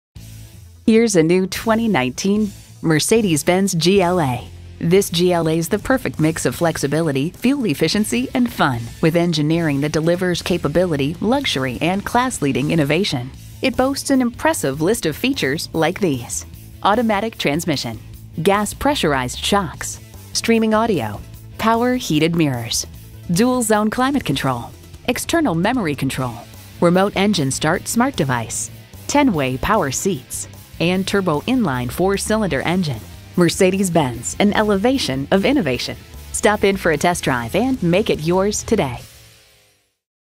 New 2019 Mercedes-Benz GLA Silver Spring MD Washington-DC, MD J90165 - SOLD
Year: 2019
Make: Mercedes-Benz
Model: GLA
Trim: GLA 250
Engine: 2.0L I4 DI Turbocharged
Transmission: 7-Speed Automatic
Color: White
Interior: Black
Stock #: J90165
VIN: WDCTG4GB3KJ552543

Address:
3301 Briggs Chaney Rd.